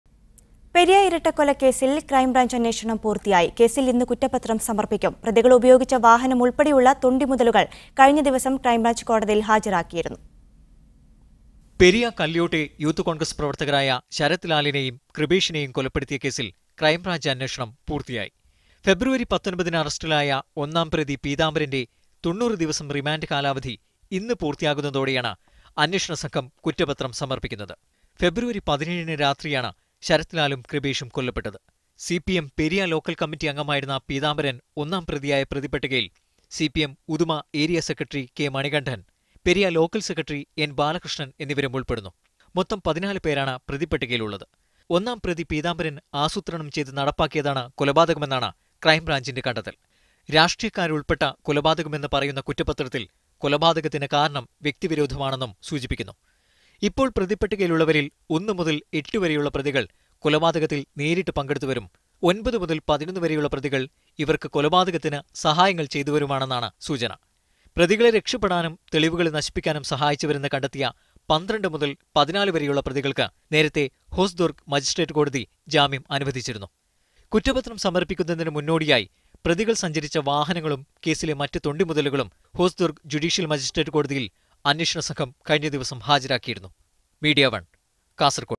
പെരിയ ഇരട്ട കൊലക്കേസ്; ക്രൈംബ്രാഞ്ച് അന്വേഷണം പൂർത്തിയായി | Kasrgode Periya Murder
പെരിയ ഇരട്ട കൊലക്കേസിൽ ക്രൈംബ്രാഞ്ച് അന്വേഷണം പൂർത്തിയായി. കേസില്‍ ഇന്ന് കുറ്റപത്രം സമര്‍പ്പിക്കും. പ്രതികള്‍ ഉപയോഗിച്ച വാഹനങ്ങളുളുള്‍പ്പടെയുള്ള തൊണ്ടിമുതലുകള്‍ കഴിഞ്ഞ ദിവസം ക്രൈംബ്രാഞ്ച് കോടതിയില്‍ ഹാജരാക്കിയിരുന്നു.

Prime shows from the channel include Special Edition, News Theatre,  Zero Hour,  View Point, Kerala Summit, Beyond the Headlines and many more.